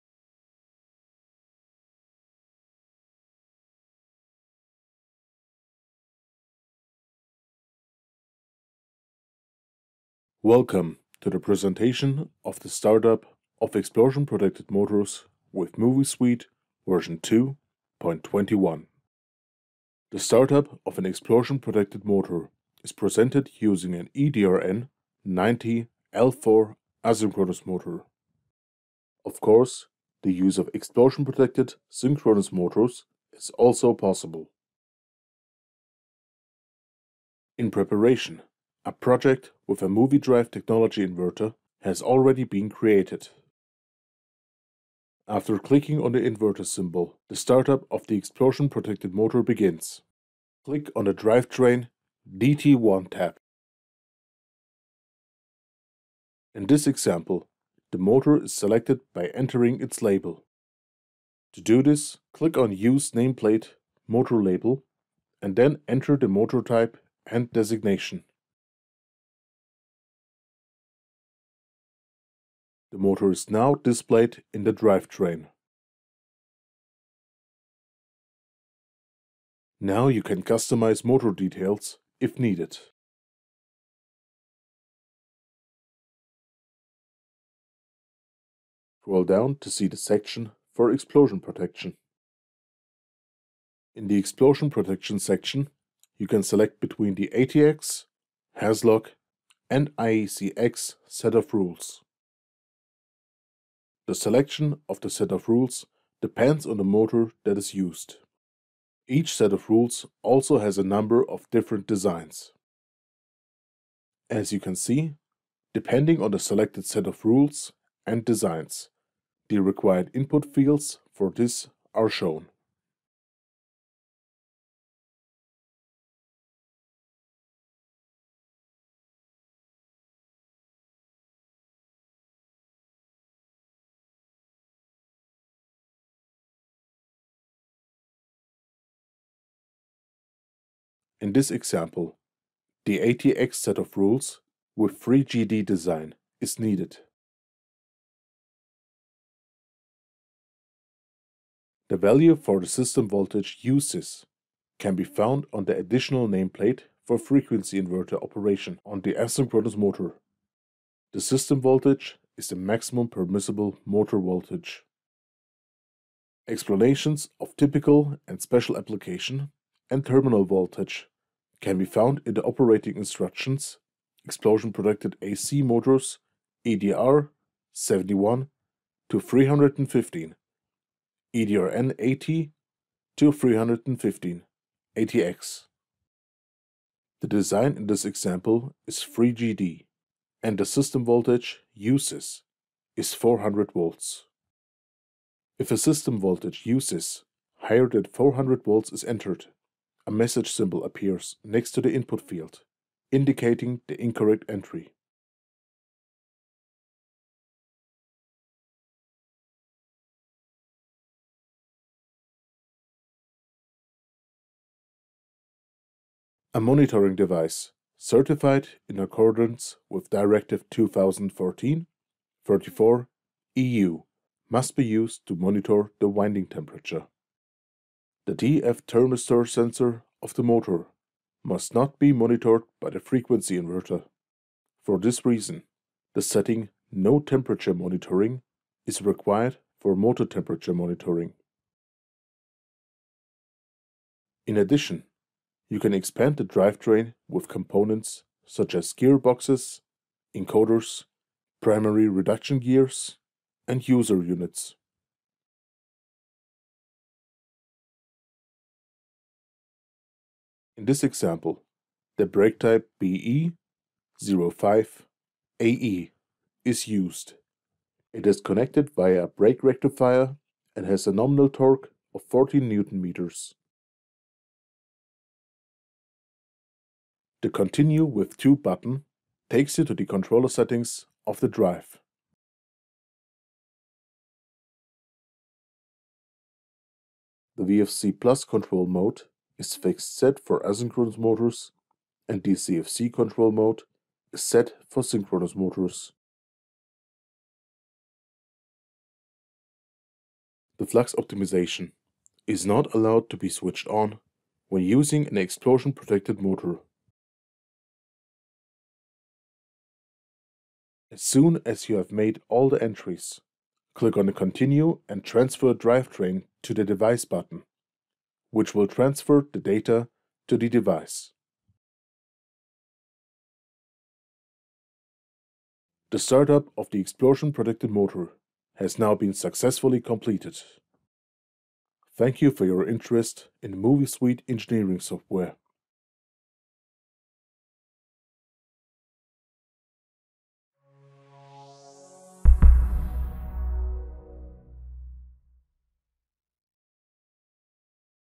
MOVISUITE v2.21 - Start up of explosion protected motors
This video explains how to start-up an explosion-protected motor with MOVISUITE software version 2.21.
The commissioning of explosion-proof motors is possible with synchronous and asynchronous motors.  ATEX, Hazloc and IECEx rules can be used.